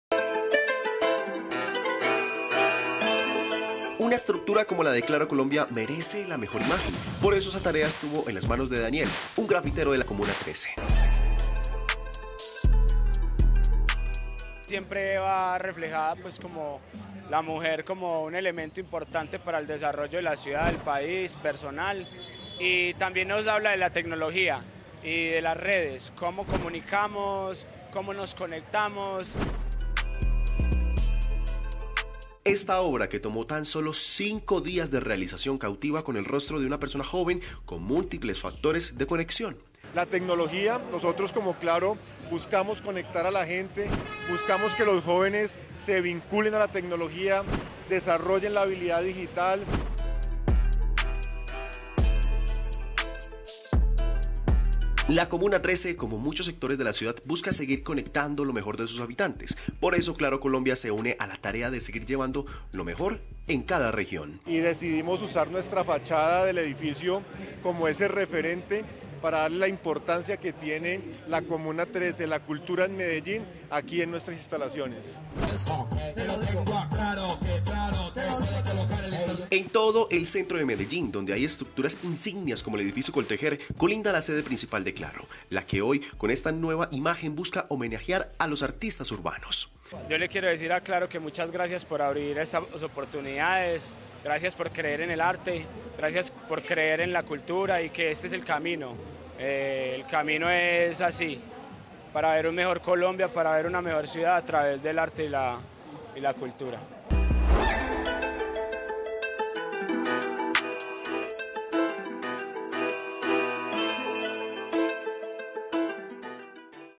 Red+ | Claro Colombia busca conectar lo mejor de Medellín y de sus jóvenes con grafitis
Claro Colombia entregó a los paisas un grafiti realizado por uno de los fundadores del grafitour de la comuna 13, con esto, se busca enviar un mensaje de apoyo a la cultura urbana como factor importante en el desarrollo de Medellín…